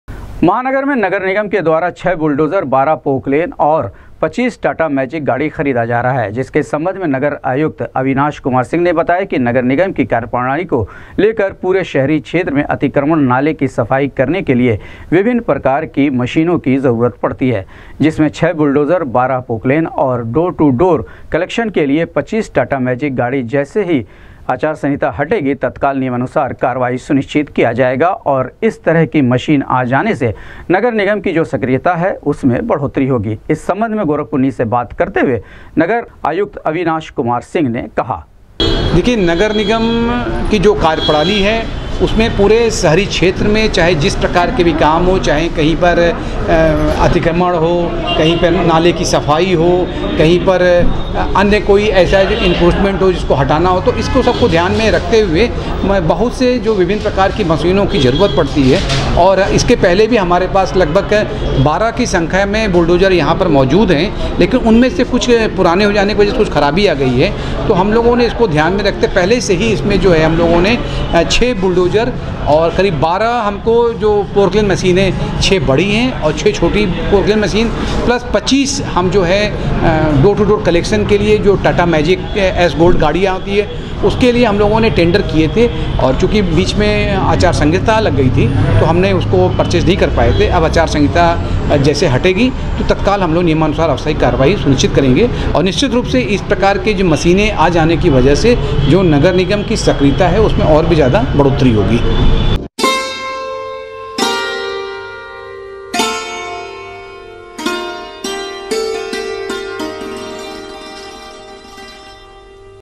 नगर निगम के द्वारा खरीदी जा रही है 6 बुलडोजर 12 पोकलेन और 25 टाटा मैजिक गाड़ी , नगर आयुक्त अविनाश
नगर निगम के द्वारा खरीदी जा रही है 6 बुलडोजर 12 पोकलेन और 25 टाटा मैजिक गाड़ी , नगर आयुक्त अविनाश कुमार सिंह ने कहा, इस खरीदारी से नगर निगम की सक्रियता में होगी बढ़ोत्तरी